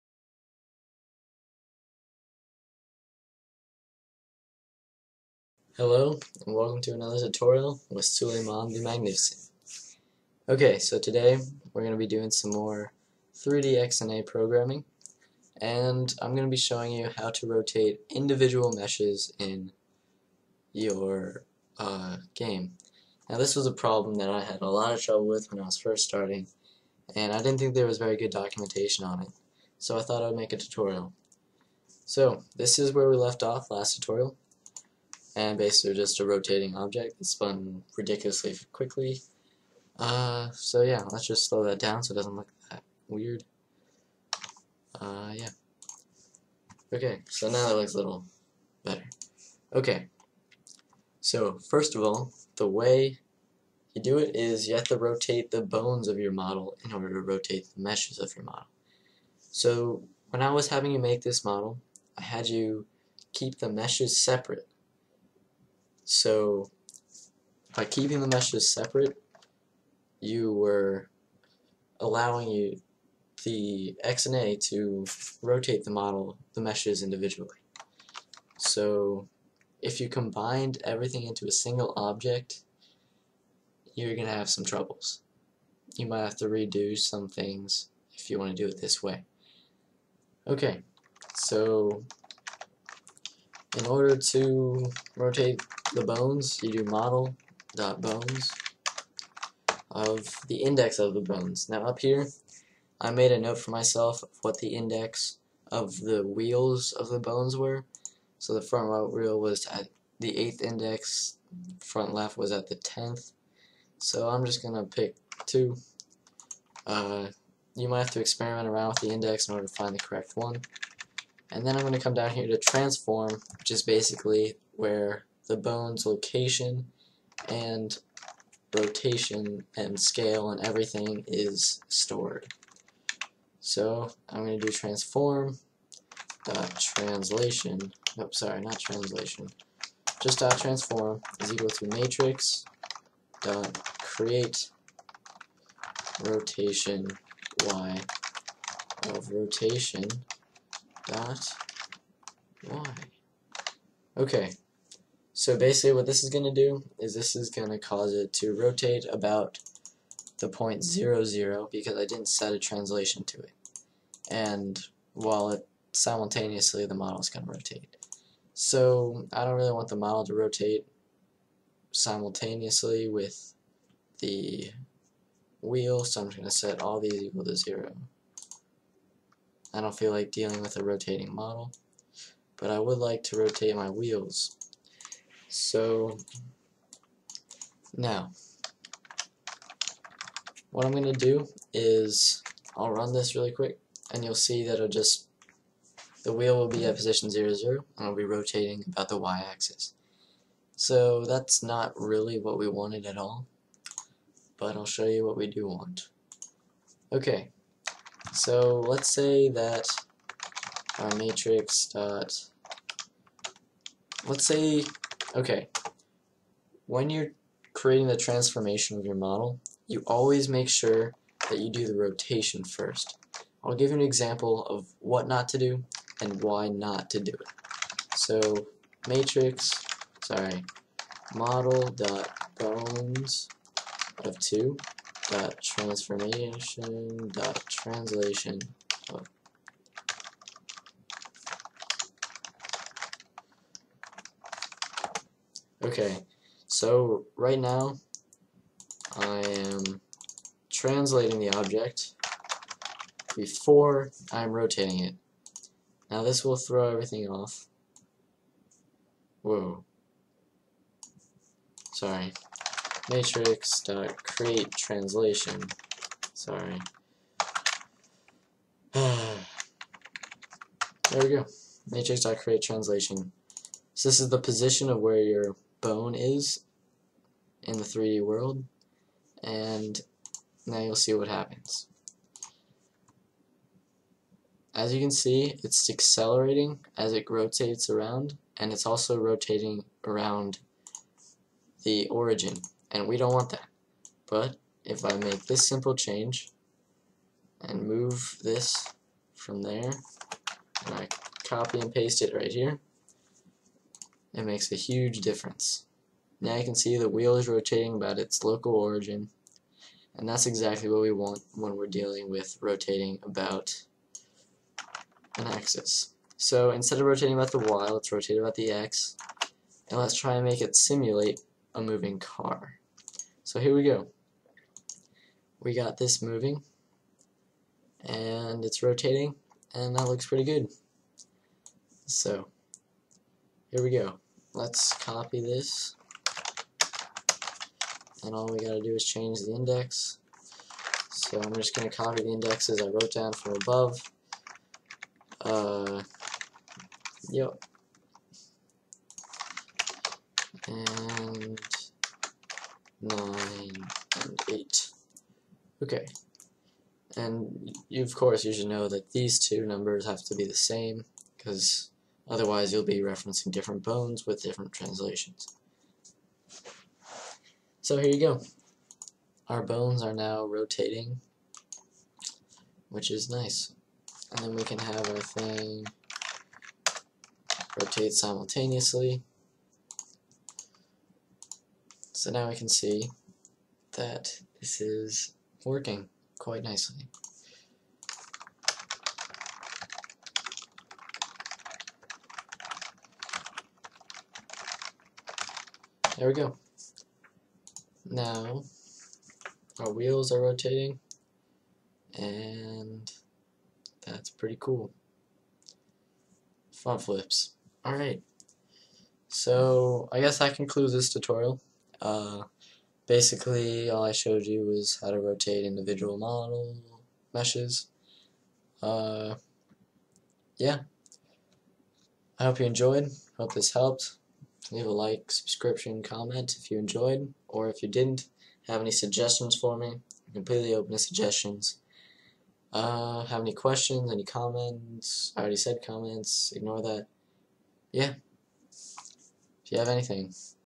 XNA Game Studio 4.0 3D Tutorial #3 - Rotating Individual Meshes
A basic tutorial showing you how to rotate individual meshes in your model.

Please comment questions or problems you have with the topic this tutorial covers, or any other topic I can help you with.

Here is a link to my dropbox page for any files